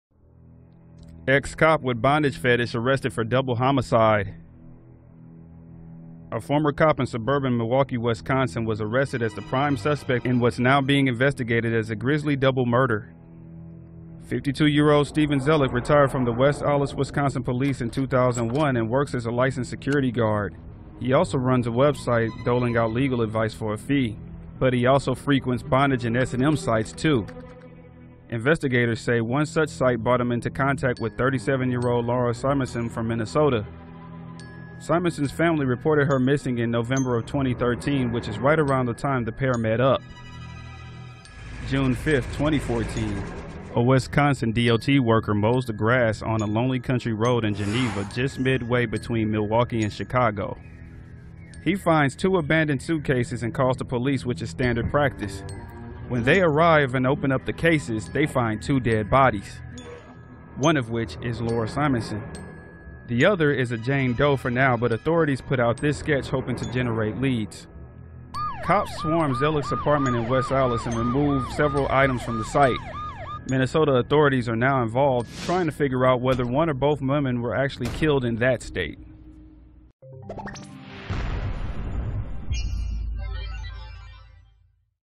Ex-cop Steven Zelich main suspect in double murder
A former cop in suburban Milwaukee, Wisconsin, was arrested as the prime suspect in what's now being investigated as a grisly double murder.

Steven Zeilich, retired from the West Allis Wisconsin police in 2001, and works as a licensed security guard. He also runs a website doling out legal advice for a fee. But he also frequented bondage and S&M sites as well.

Investigators say one such site brought him into contact with 37 year-old Laura Simonson from Minnesota.

Simonson's family reported her missing in November of 2013, which is right around the time the pair met up.

June 5th, 2014. A Wisconsin DOT worker is mowing the grass alongside a road in Geneva, midway between Chicago and Milwaukee. He finds two abandoned suitcases and calls the police, standard practice. When the police arrive and opened the case they found 2 bodies. Laura Simonson, and a 'Jane Doe'. Authorities put out a sketch hoping to generate leads on the second woman's identity.

Cops swarmed Zelich's apartment in West Allis, interviewing others in the building and removing several items from the site. Minnesota authorities are now involved, trying to figure out whether one or both women were actually killed in that state.